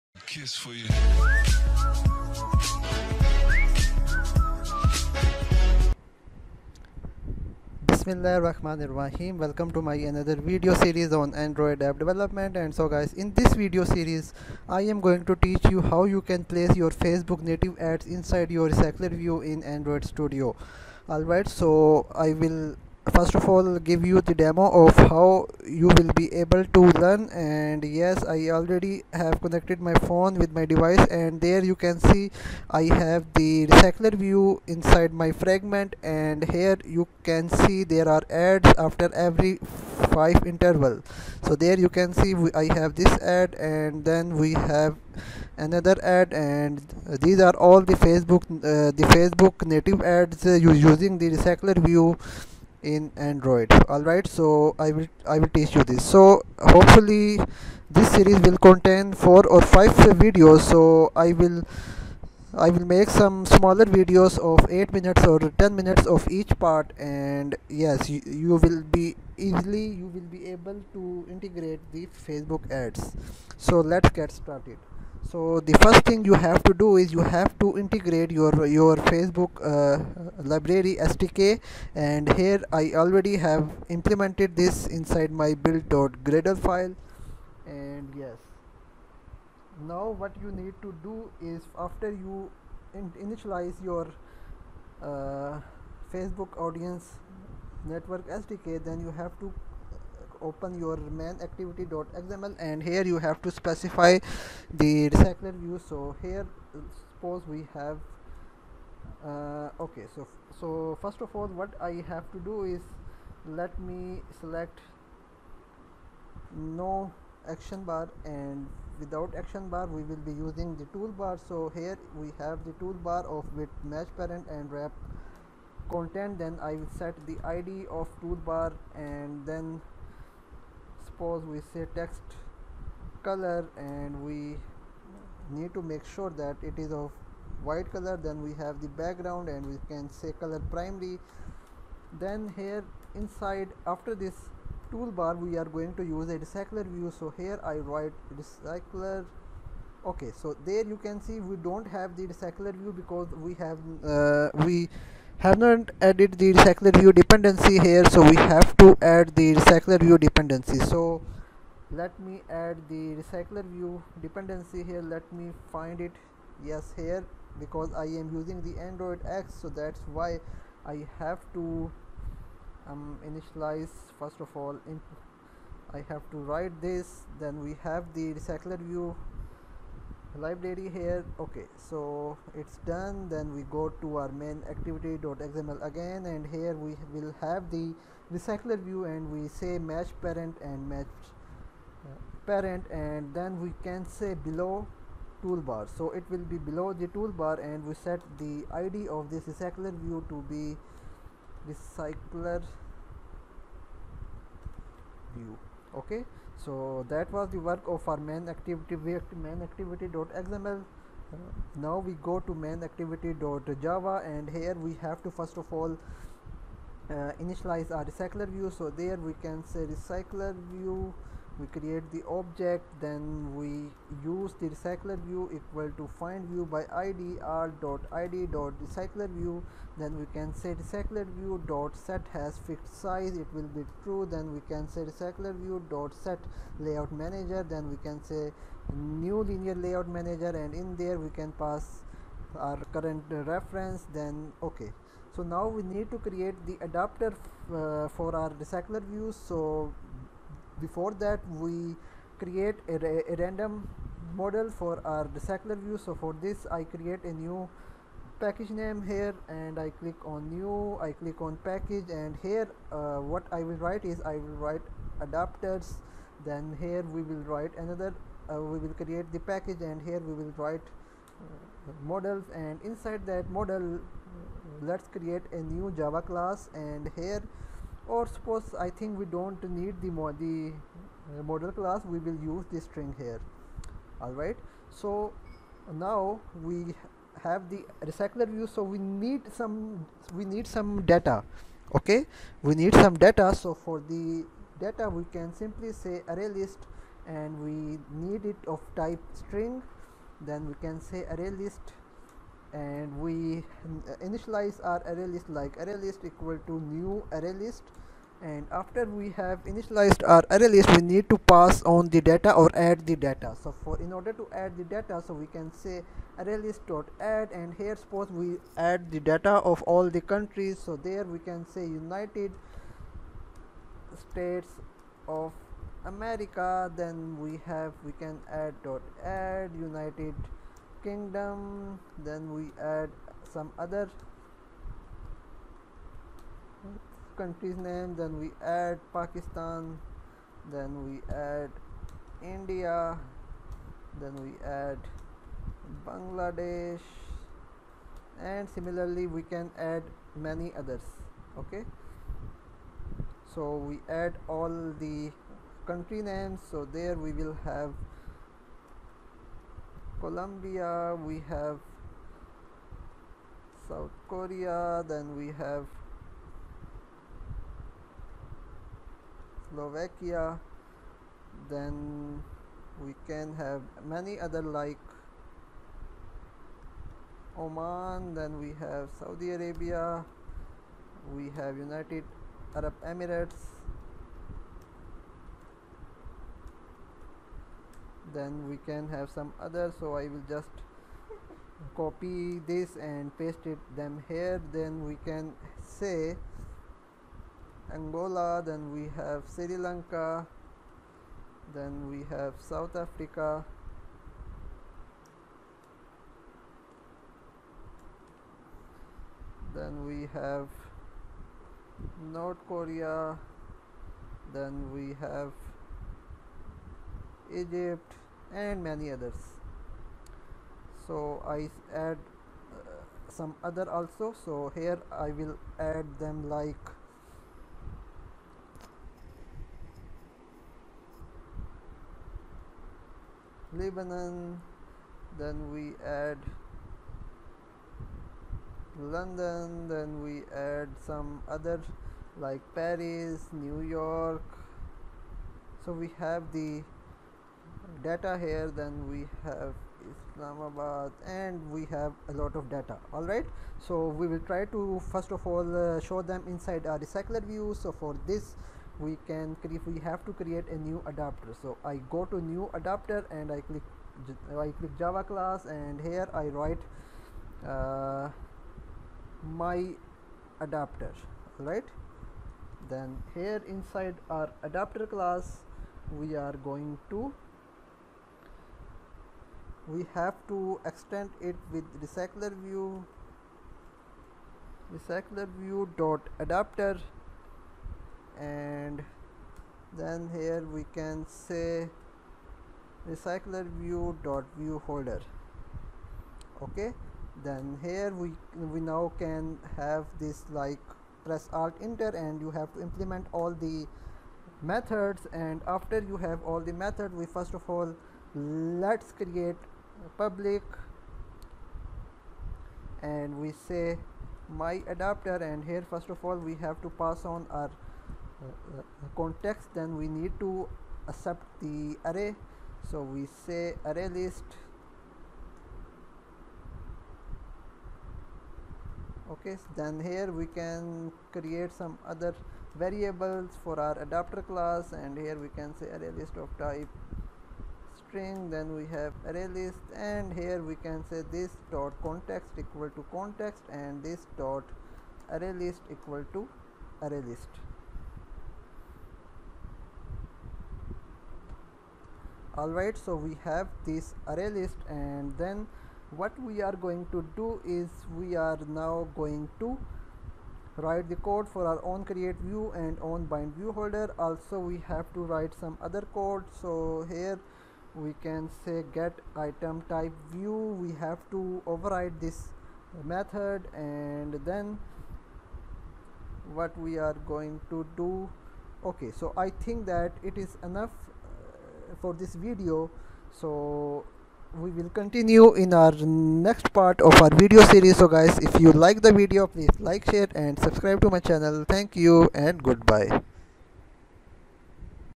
Facebook Native Ads with RecyclerView in Android Studio Tutorial PART 01
Hi developers.
In this video series, I am going to teach you how you can integrate facebook native ads in recyclerview using android studio. So you will learn about the facebook sdk in general and the native ads which will appear inside the recyclerview.